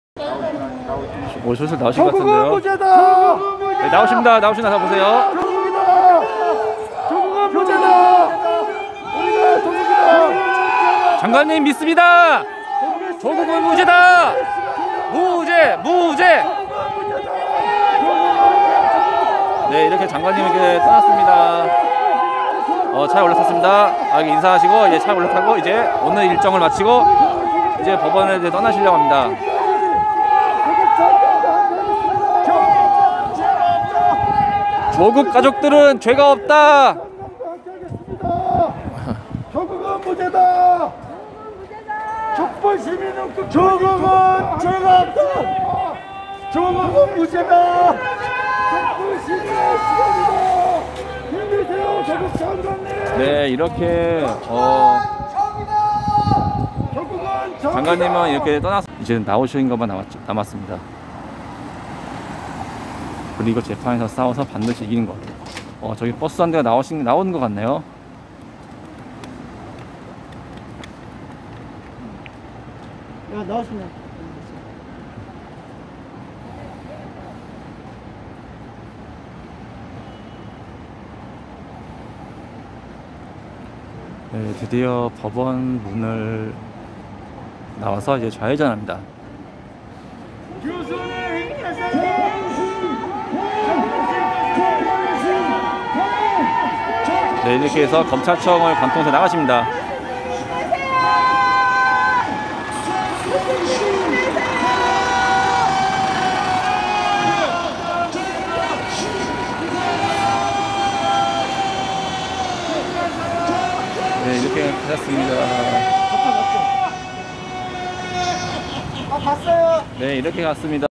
조국 장관님과 정경심 교수님이 재판 마치고 가시는 모습.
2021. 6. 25

서울중앙지법.

오늘 재판 받느라 고생하셨습니다.
깨시민들의 응원을 듣고 힘내시고 반드시 승리하시길 기원합니다.  반드시 진실은 이긴다.

자발적 후원

농협:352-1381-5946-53 예금주:김@남(심야 k.김@초)